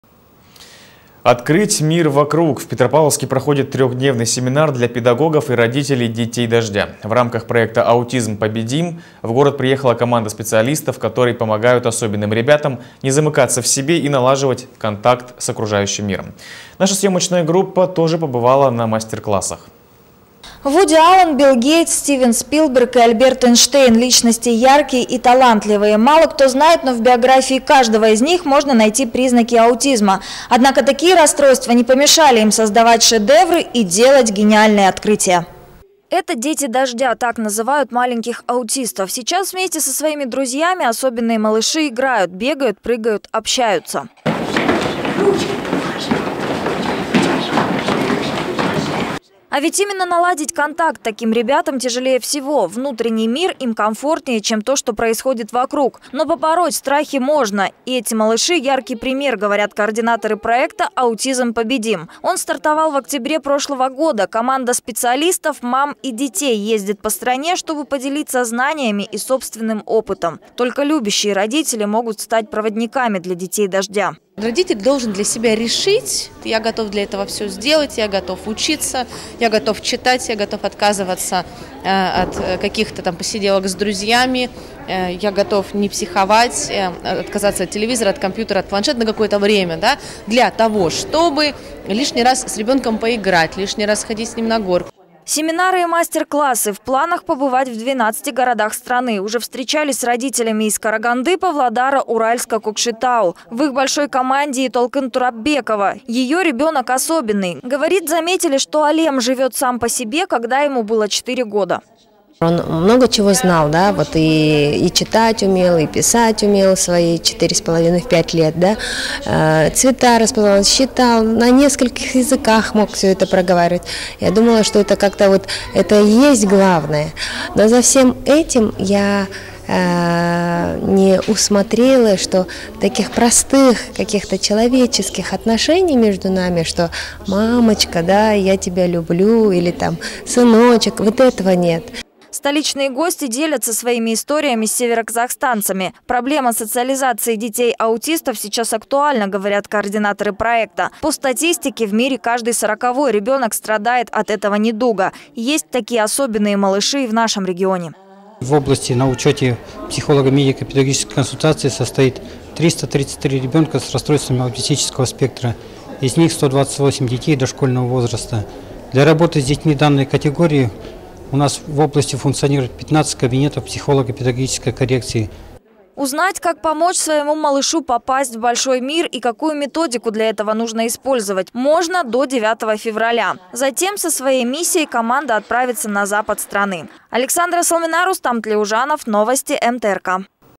Рус Семинар Аутизм
Открыть мир вокруг! В Петропавловске проходит трехдневный семинар для педагогов и родителей детей дождя. В рамках проекта "Аутизм победим" в город приехала команда специалистов, которые помогают особенным ребятам не замыкаться в себе и налаживать контакт с окружающим миром. Наша съемочная группа тоже побывала на мастер-классах.